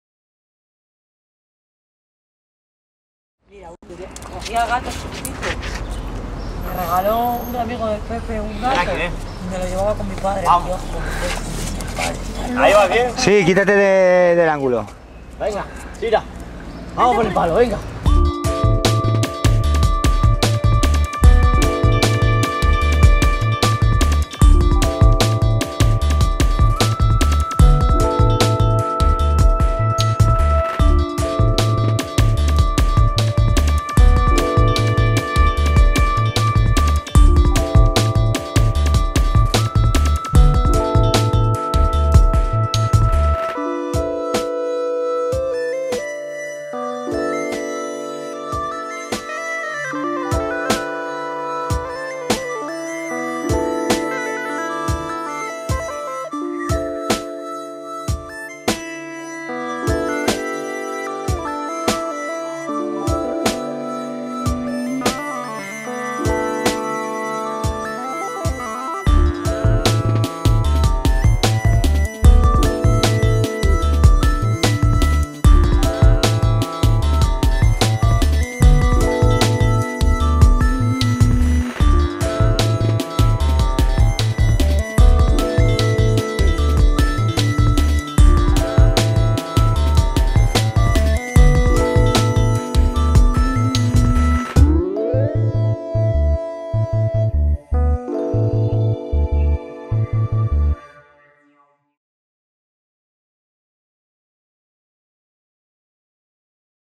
American Stafford - Saltando! & Dog Jump
Aquí tenemos a Sira una perrita Stanford, pegando saltos de 2 metros de altura, como poco. para cojer su palo...

debajo de mi casa siempre se ponen unos chavales y vi los pedazo saltos que daba la perrita y les pedi permiso un día que baje a mi perrito Krispin. y realize este video

espero sea de vuestro agrado

Muchas Gracias